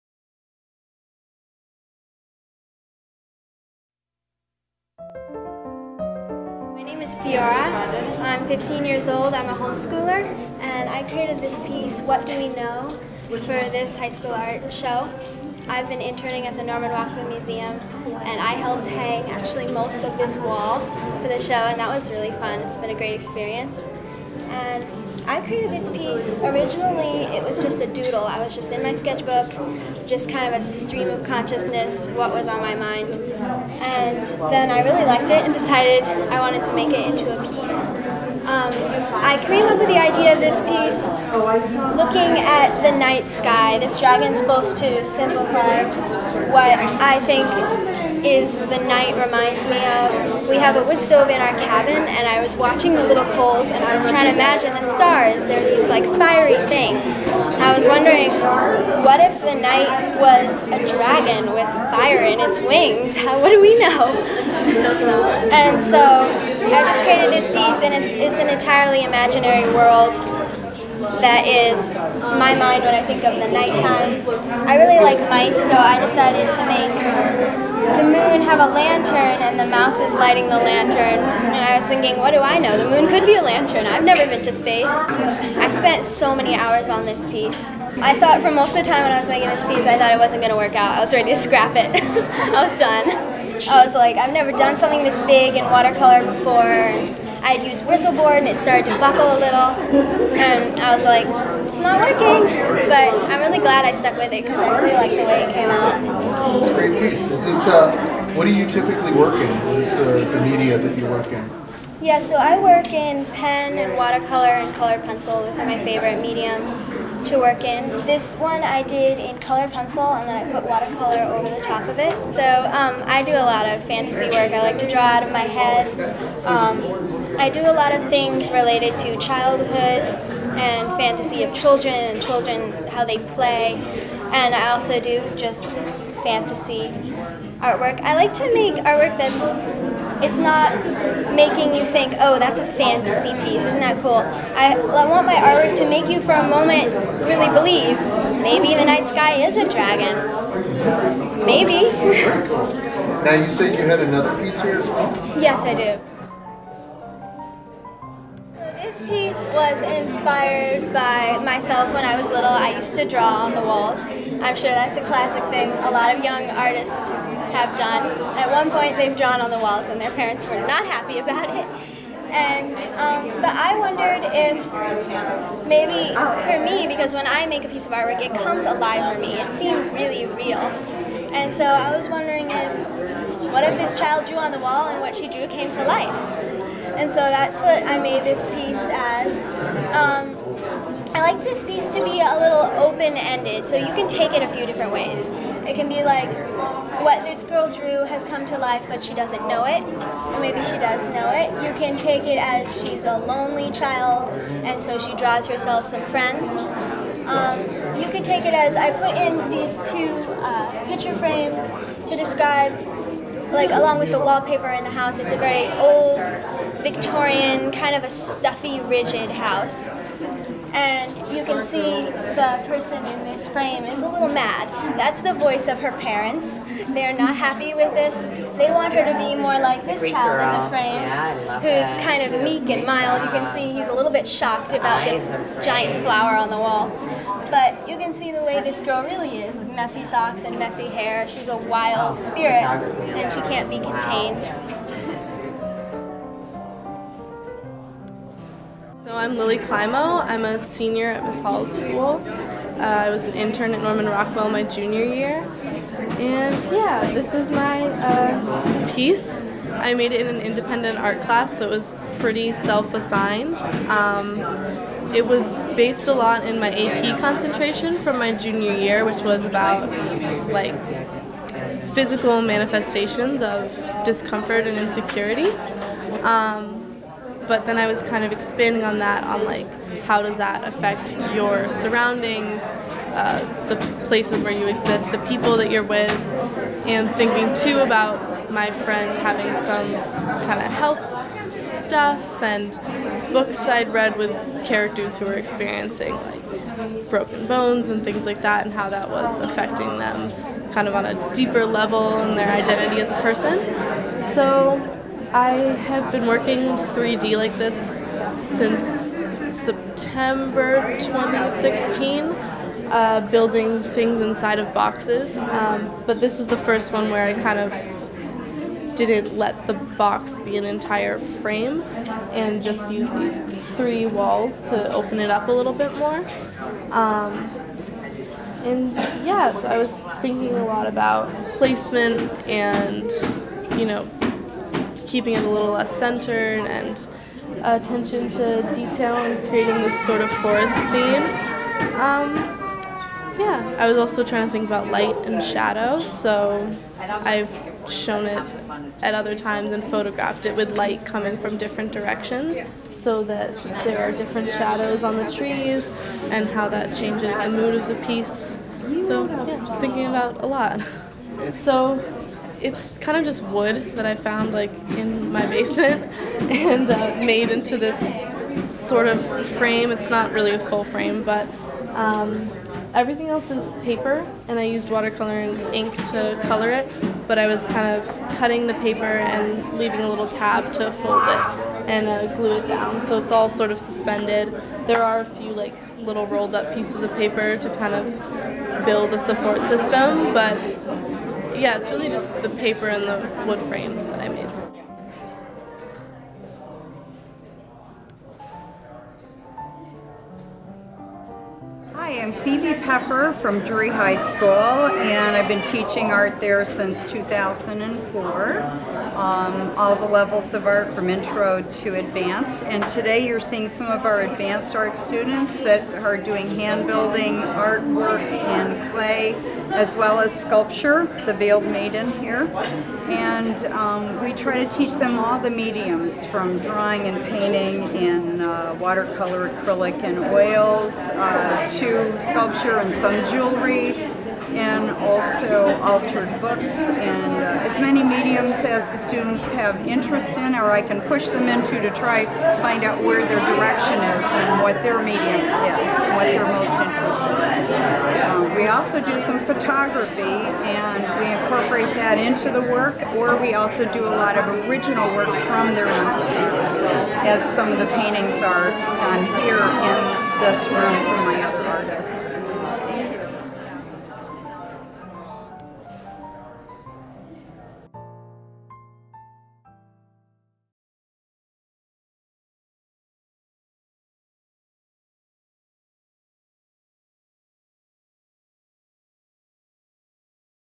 32nd Annual Berkshire County High School Art Show
February 3, 2018 through March 4, 2018

Celebrating the creative talent on display from Berkshire County high schools. Original works in all media—from painting and drawing to photography, ceramics, three-dimensional assemblage, and digital art—will be on view. Participating schools include: BART Charter School, Berkshire School, Berkshire Waldorf School, Drury High School, Lee High School, Miss Hall’s School, Monument Mountain Regional High School, Mount Everett Regional High School, and Wahconah Regional High School.